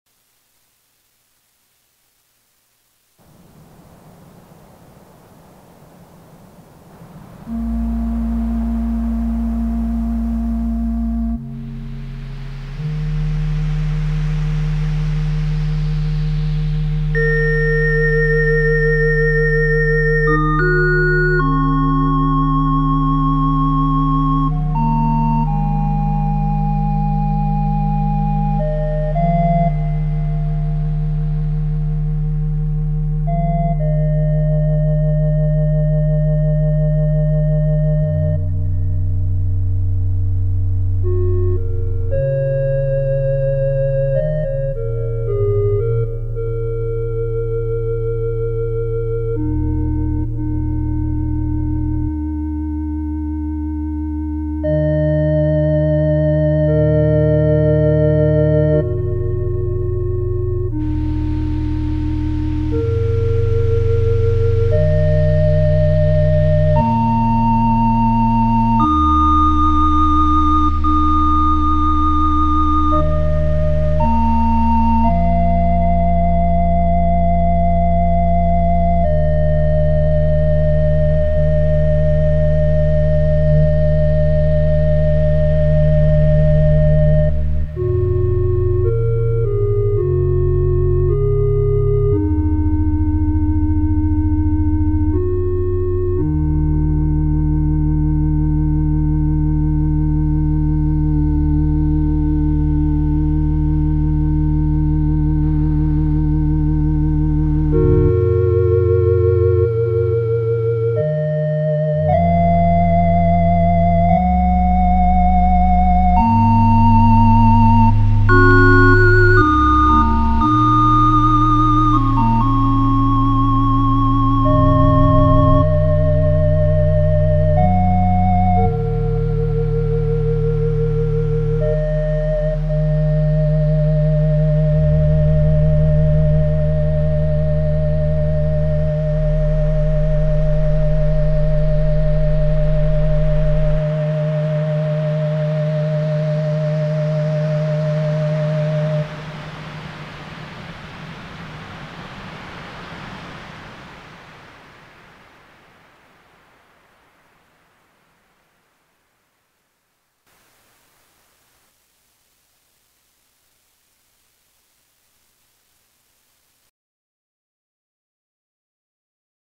ocarina remembrance
Remembrance for the two police officers killed in Brooklyn, New York.
This was the second and third revenge killing stemming from the
ferguson riots (the first being of Zemir Begic.)